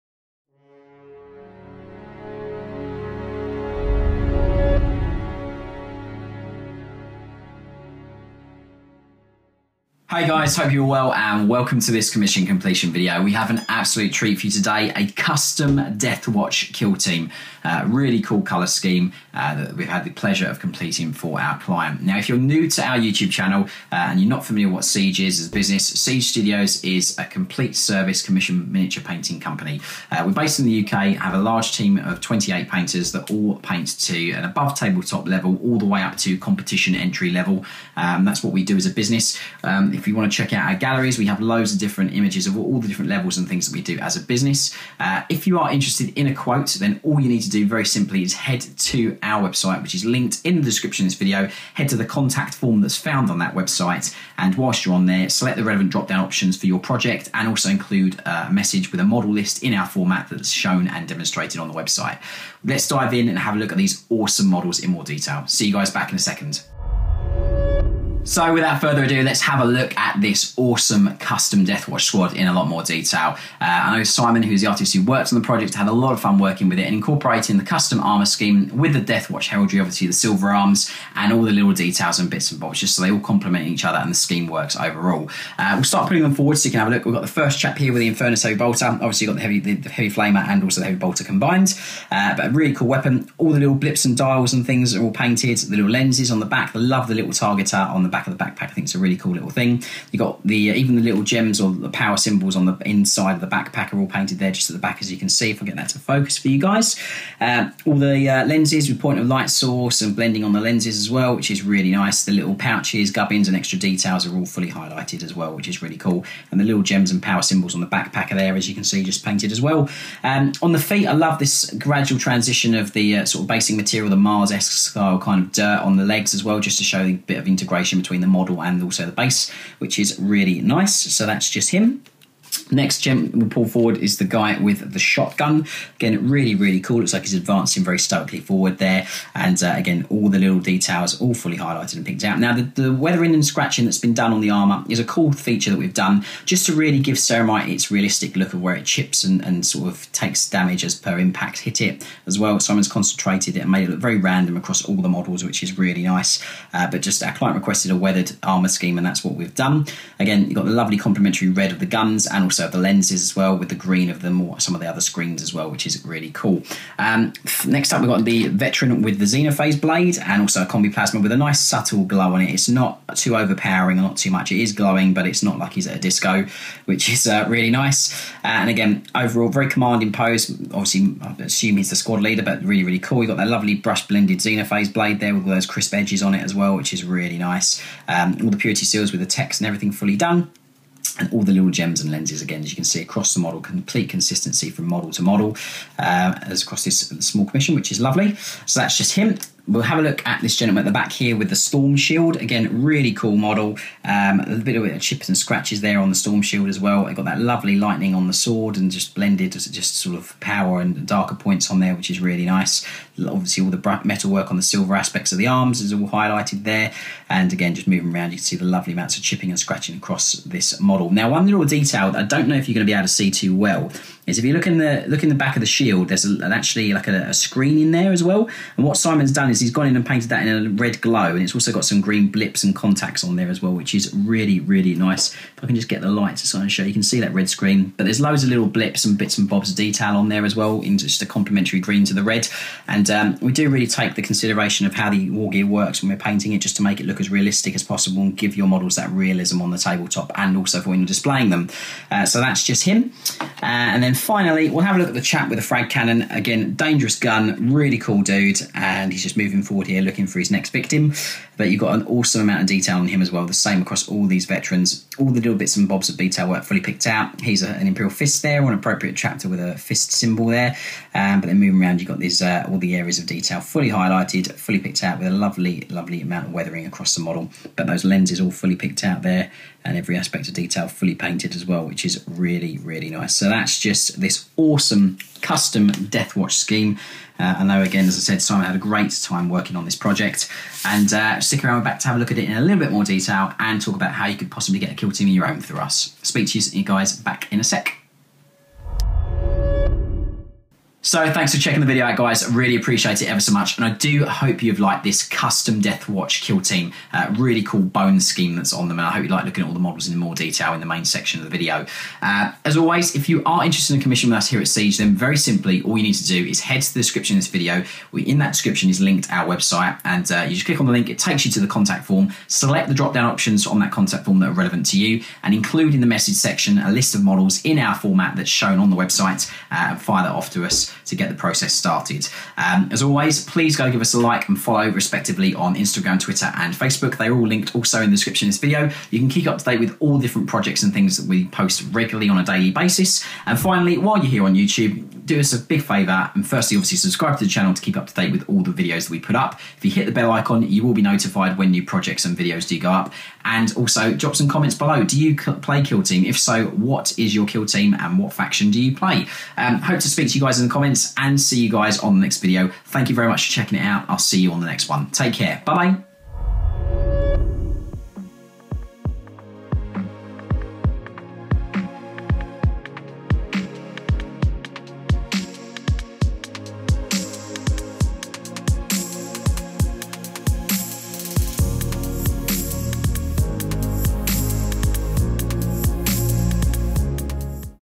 CUSTOM Deathwatch Kill Team Showcase Painted by Siege Studios
For our showcase today we have this great Deathwatch Kill Team for Warhammer 40k 9th Edition, completed to our Silver painting level.

We guarantee a premium service with unrivalled painting quality, attention to detail and client experience.